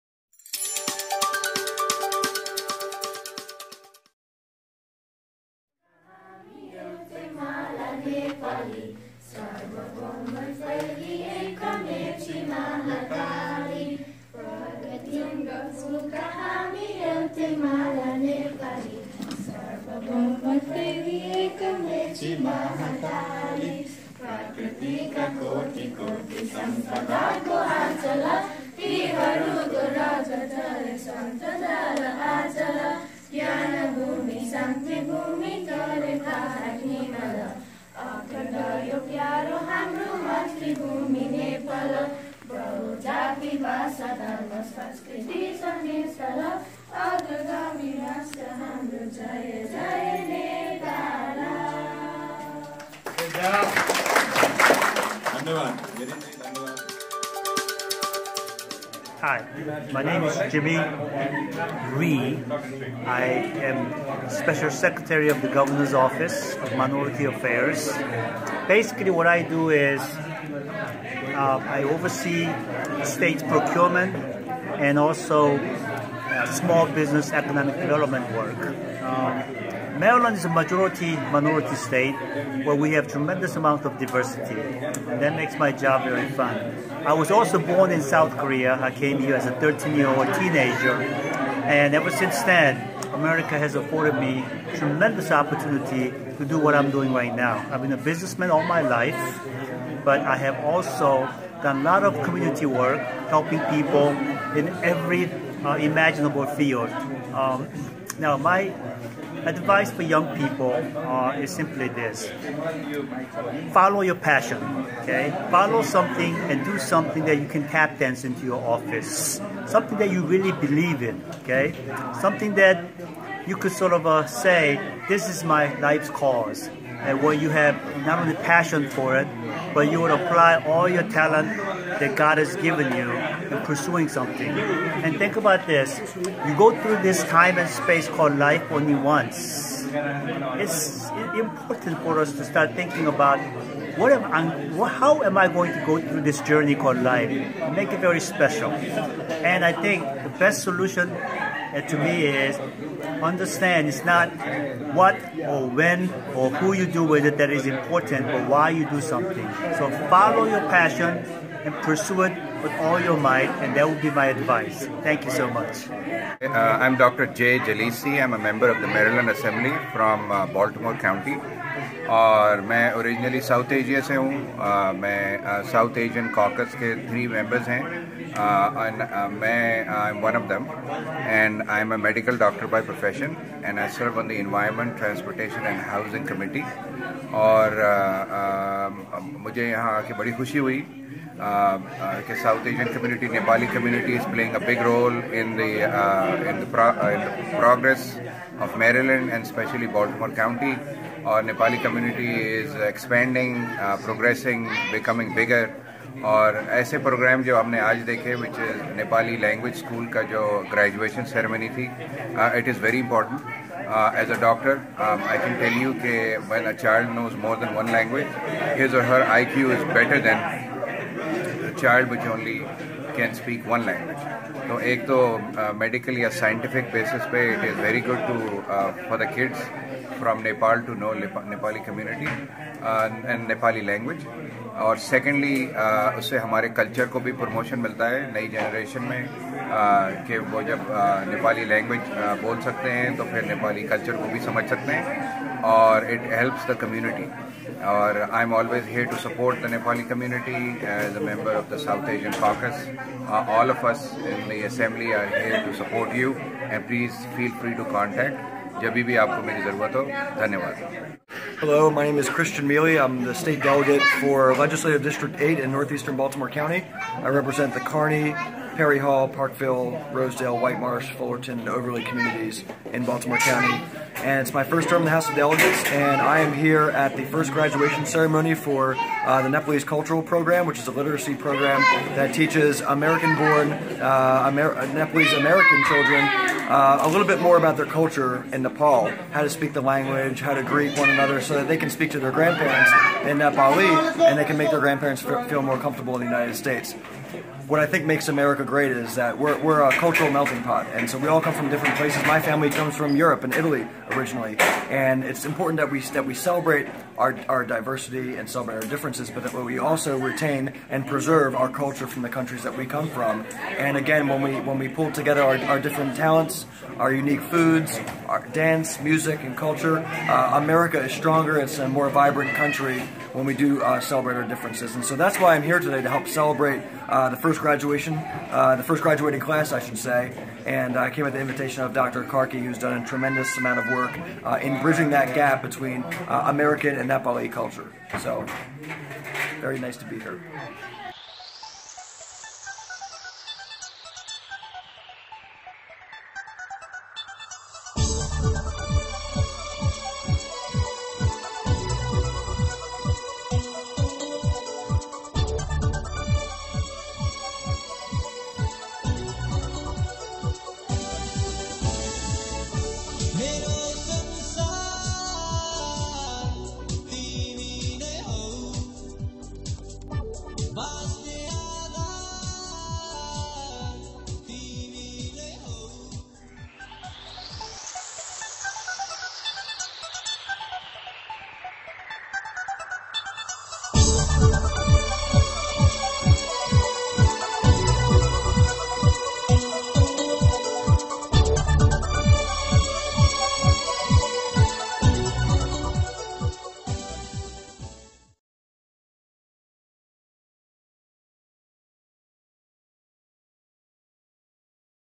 Graduation Day 2016 - Nepali Language School, Baltimore, MD
Graduation Day - 2016,
Nepali Language School, Baltimore, MD
Video from the Graduation Ceremony at the Nepali Language School.

Few valuable words from the Guest of Honor and Member of House of Delegates Jay Jalisi, Maryland Secretary for Minority Affairs Jimmy Rhee and Delegate Christian Miele.
Shyam Karki, Bheem Kattel, Shrijana Khanal, Kumar Karki, Ram Bhandari, Rabina Thapa, Shiva Malla, Satish Chapagain, Srijan Tamrakar

Camera/Editing: Srijan Tamrakar

concept & design: Srijan, 2016